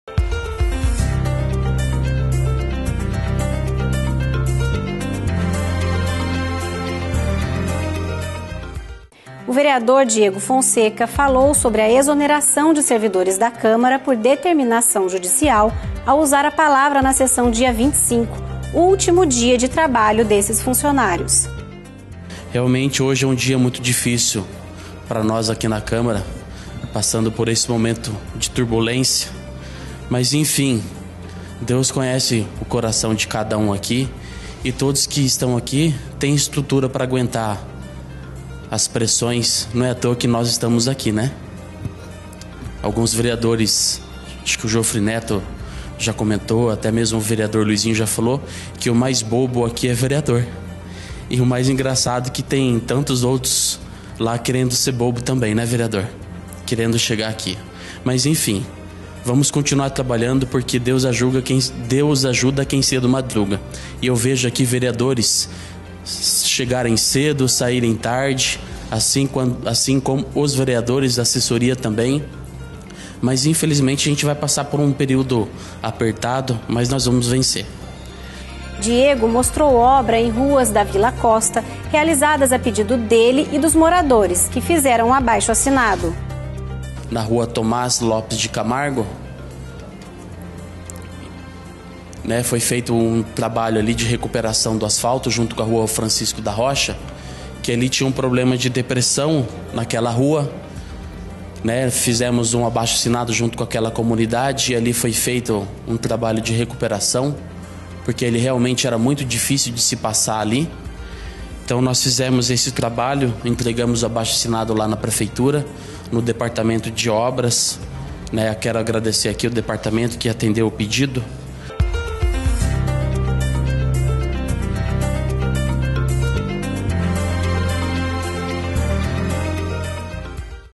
Minuto Legislativo - Ver. Diego Fonseca - 78ª Sessão Ordinária
O vereador Diego Fonseca falou sobre a exoneração de funcionários da Câmara por determinação judicial, ao usar a palavra na sessão dia 25, último dia de trabalho destes servidores.